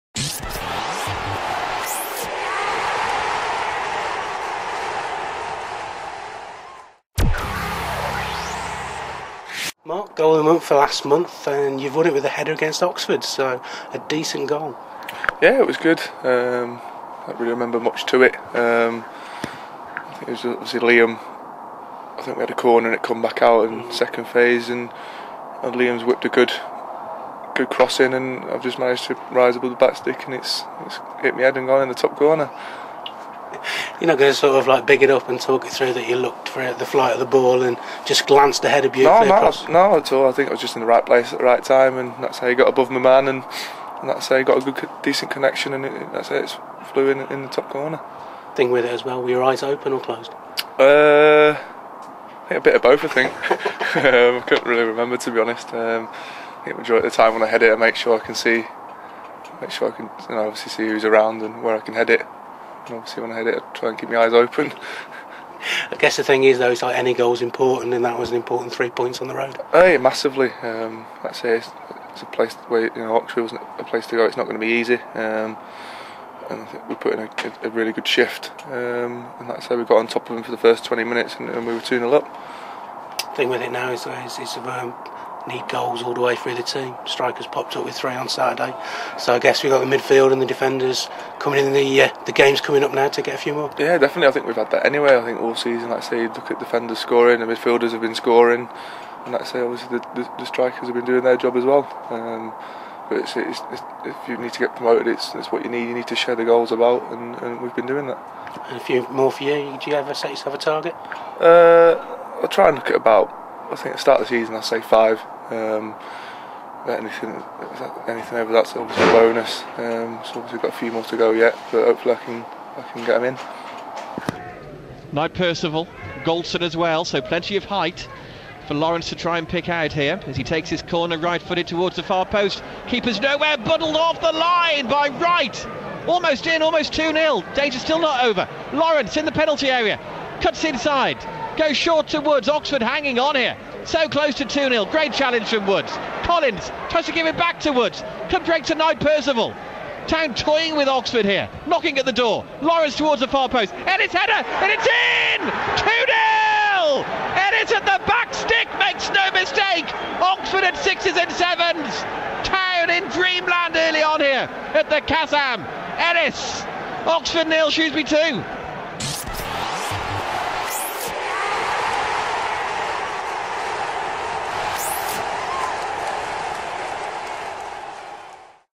Goal of the Month December winner - Town TV
Mark Ellis's goal away to Oxford United was your winner in the December Goal of the Month